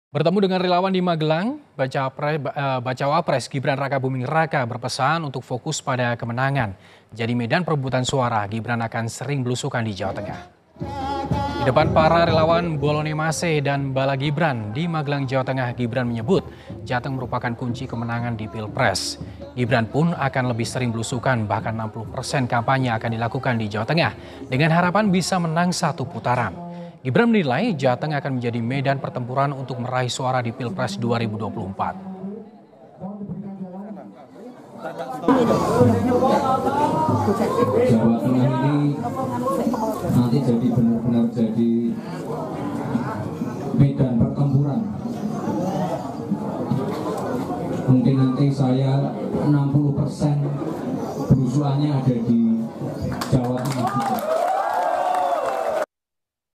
Gibran Ungkap Strategi untuk Merebut Suara di Jawa Tengah
KOMPAS.TV - Bertemu dengan relawan di Magelang, Bacawapres Gibran Rakabuming Raka berpesan untuk fokus pada kemenangan. Jadi medan perebutan suara Gibran akan sering blusukan di Jateng.

Di depan para relawan Bolone Mase dan Bala Gibran di Magelang, Jawa Tengah, Gibran menyebut Jateng merupakan kunci kemenangan di pilpres.

Gibran pun akan lebih sering blusukan bahkan 60 persen kampanye akan dilakukan di Jateng dengan harapan bisa menang satu putaran.

Gibran menilai Jateng akan menjadi medan pertempuran untuk meraih suara di Pilpres 2024.